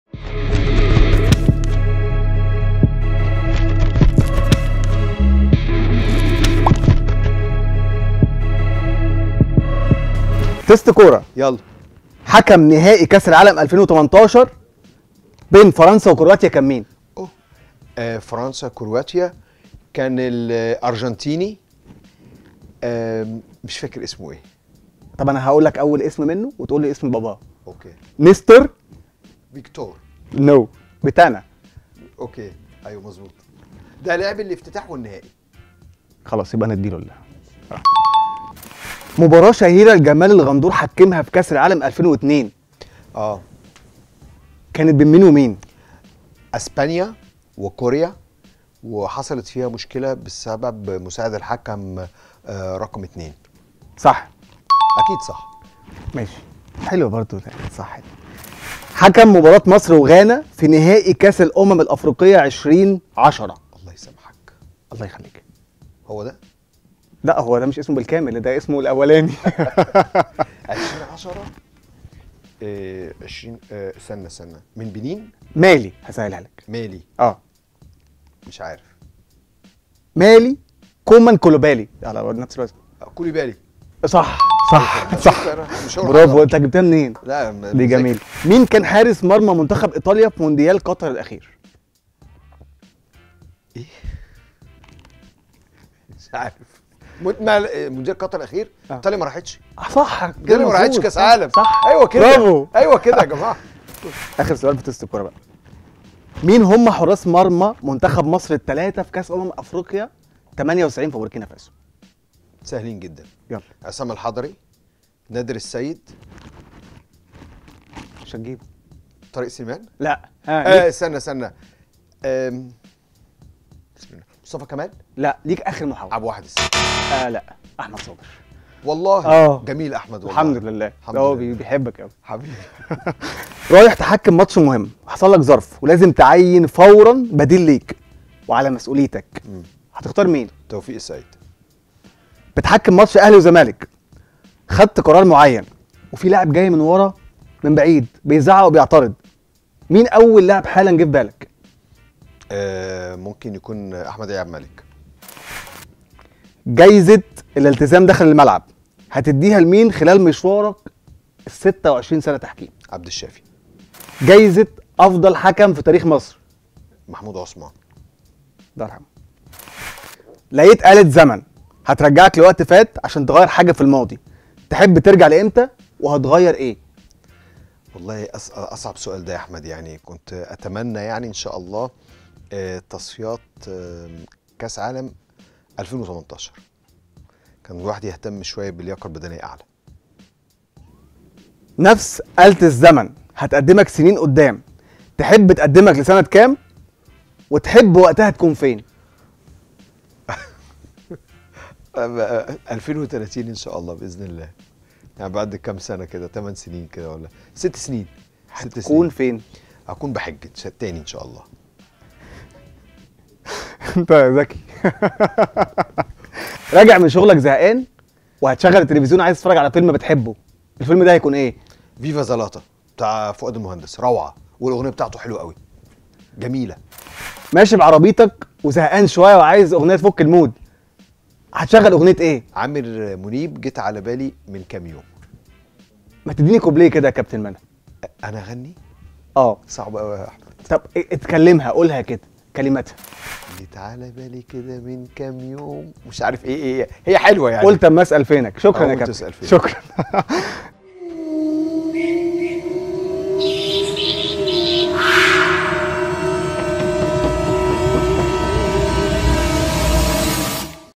لو اخترت تعين حكم مكانك هتختار مين.. وتدي لمين جايزة الالتزام داخل الملعب؟.. شوف إجابات سمير عثمان
لو اخترت تعين حكم مكانك هتختار مين.. وتدي لمين جايزة الالتزام داخل الملعب؟.. شوف إجابات سمير عثمان في